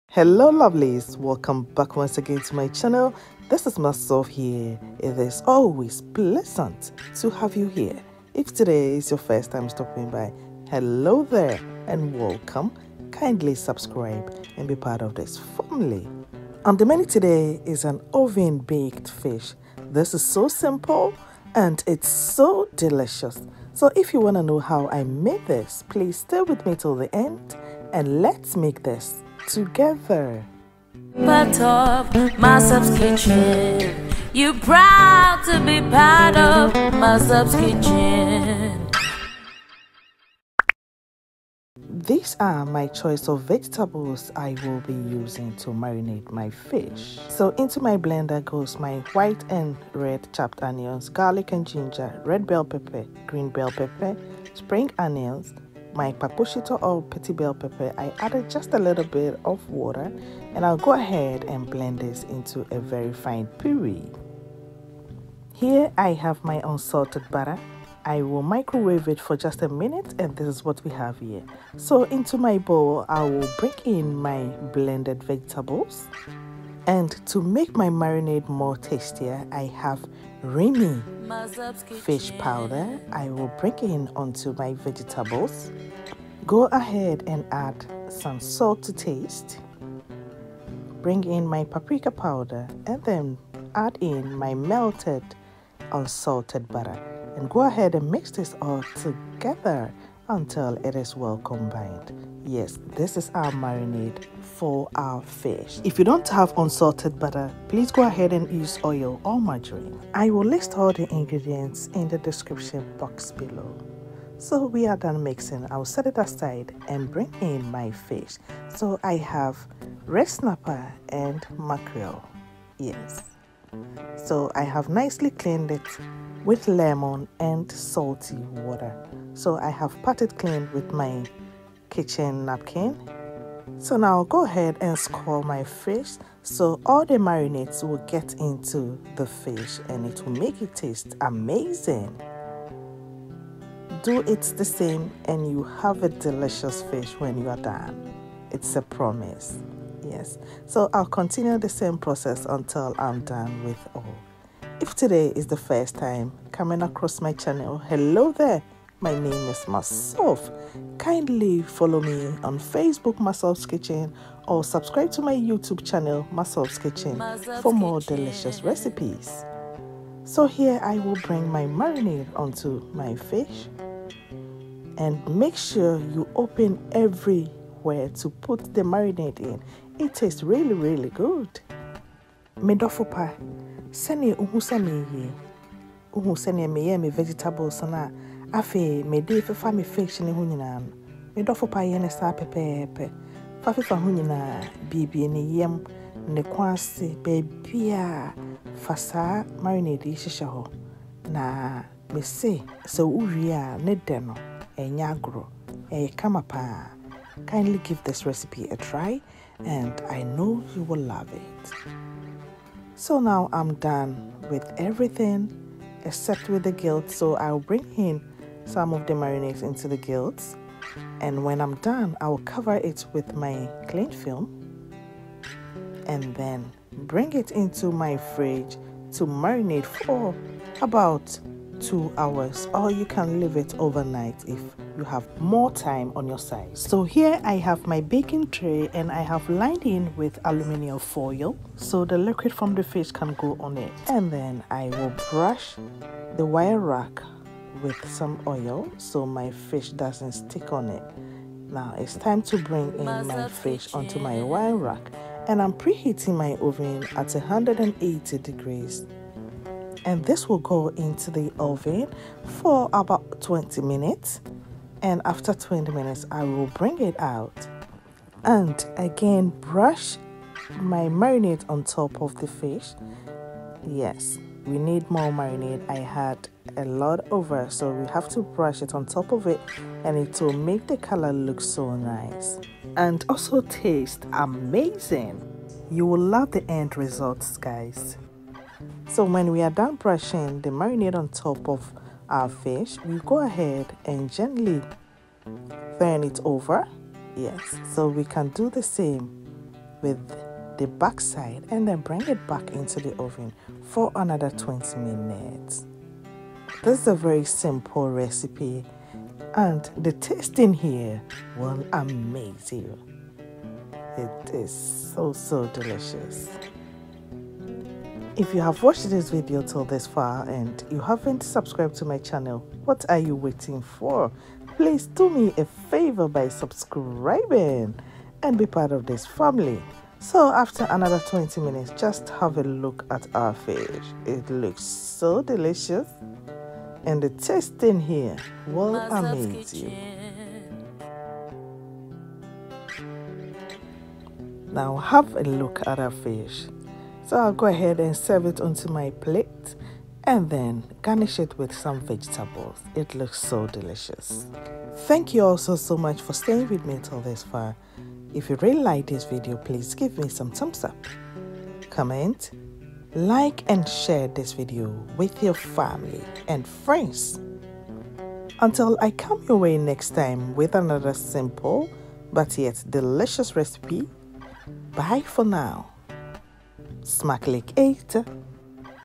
Tasty And Juicy Grilled Red Snapper And Mackerel In The Oven//@MasofsKitchen
Ingredients:
MARINADE
4 sprigs of Spring onions
1 Red bell peppers
1 Green bell peppers
1/2 Red Onion  / 1/2 white onion
Kpakpo shito( Petite bell peppers)
40g Unsalted butter / margarine
4 cloves Garlic
A thumb size Ginger
2 pack Remi fish seasoning
1 tbsp Paprika powder
2 tsp If salt

FISH
2 mackerel
2 big size Red Snapper

Other Recipes